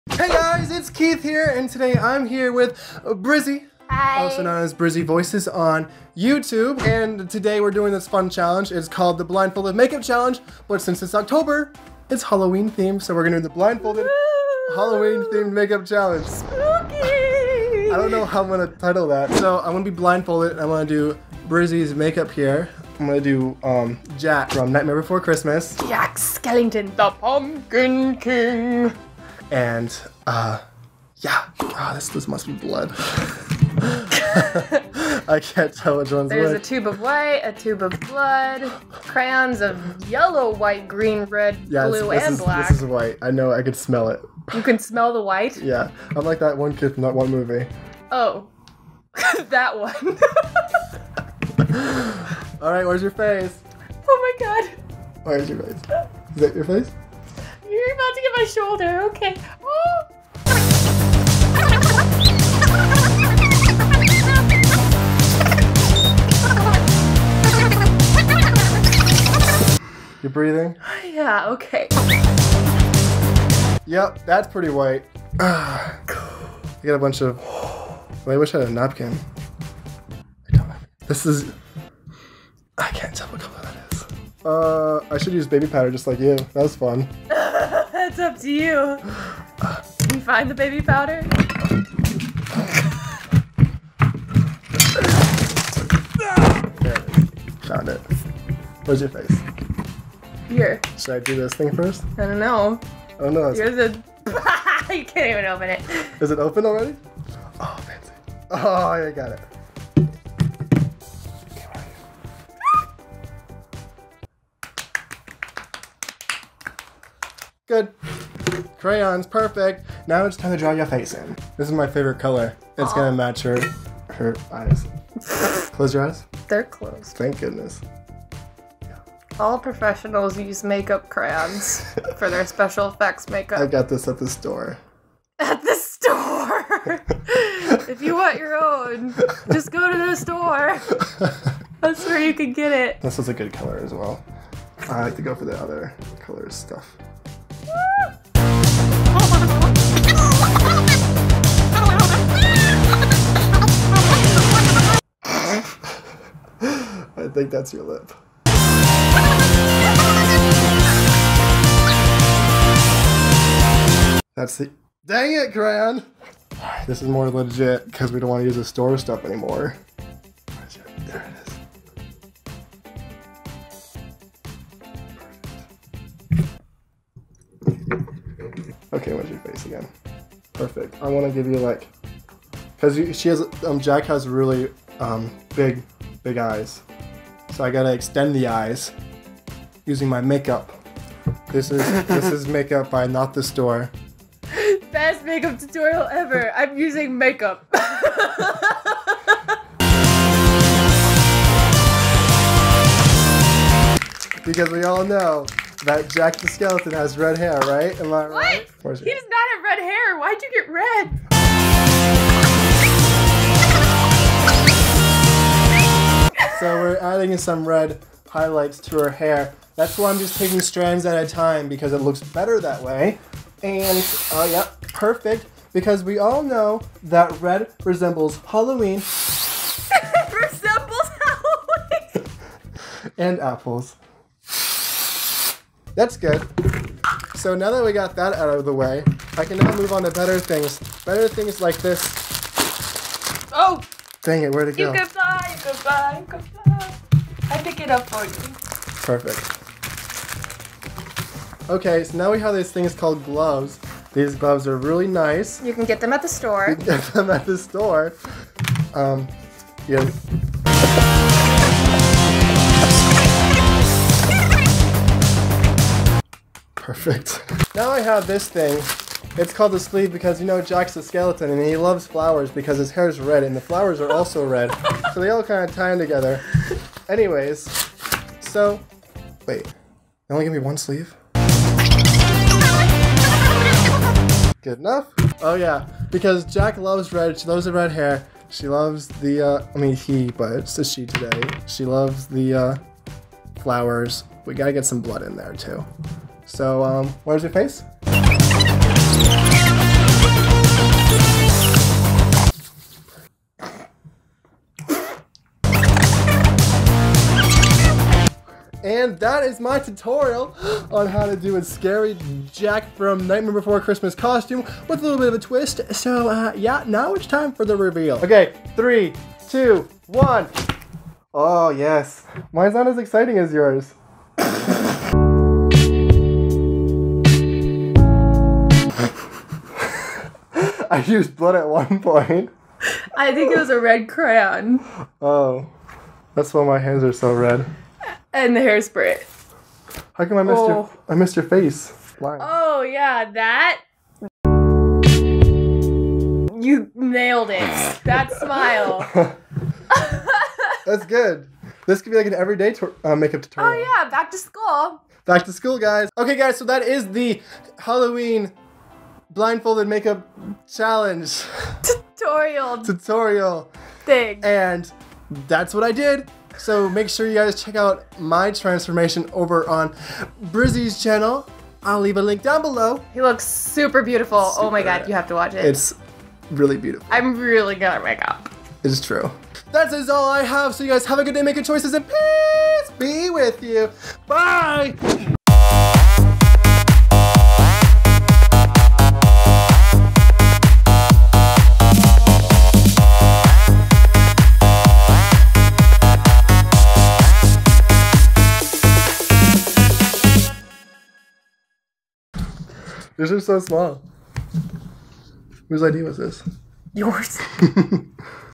BLINDFOLDED MAKEUP CHALLENGE W/ BRIZZY VOICES
Brizzy (aka Brizzy voices) and I do the blindfolded makeup challenge, but since Halloween is just around the corner we are going to shake it up a bit a do each others Halloween makeup!

STARRING:
My self... I have no friends
My name is Keith
Have a good day
Make good Choices!
and peace be with you